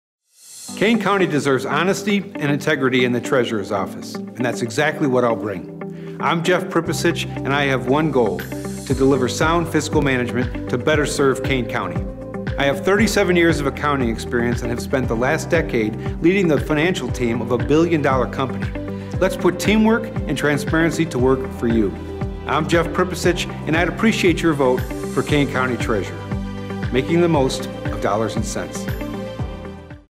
Jeff Pripusich for Kane County Treasurer | General Election: November 8, 2022
Kane County deserves honesty and integrity in the treasurer's office and that's exactly what I'll bring. I'm Jeff Pripusich and i have one goal, to deliver sound fiscal management to better serve Kane County.

I have 37 years of accounting experience and have spent the last decade leading a financial team of a billion dollar company. Let's put teamwork and transparency to work for you.

I'm Jeff Pripusich and I'd appreciate your vote for Kane County Treasurer.